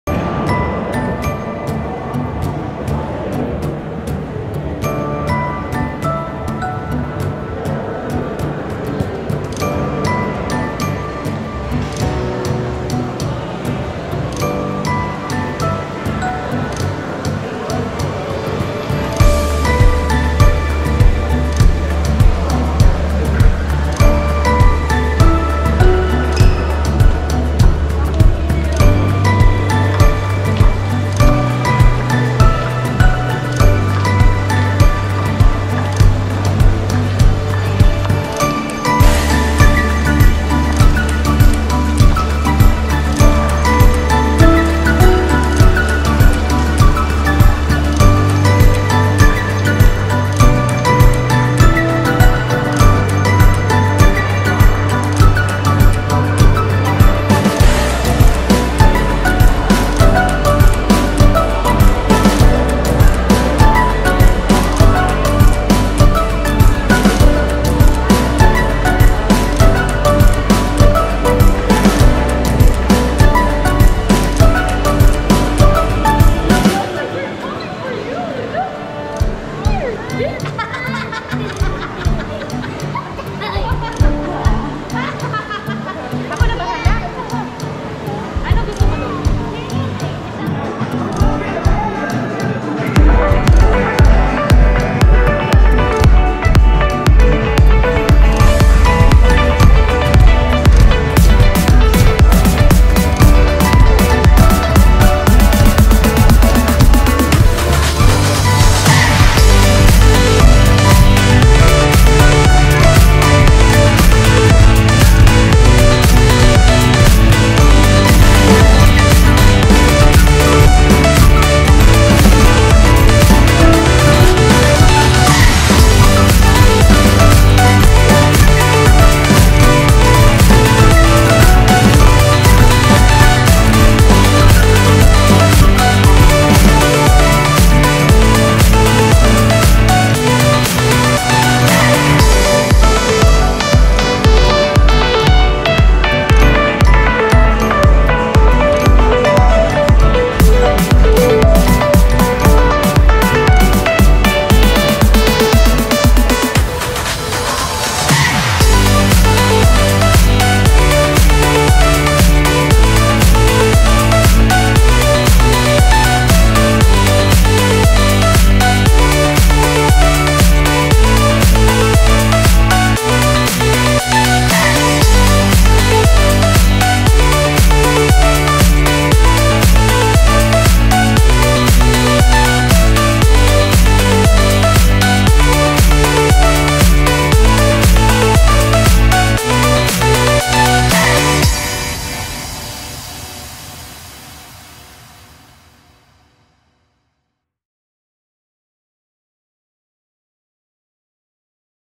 INSIDE The Atlantis Hotel in Dubai! Main aquarium wall and restaurants walk around 2022. 4K video
This is just a quick video to show the main large aquarium windows inside the Atlantis The palm. Also a look into the Gordon Ramsey Restaurant ‘Bread Street Kitchen’ Classed as a ‘Modern British Restaurant’.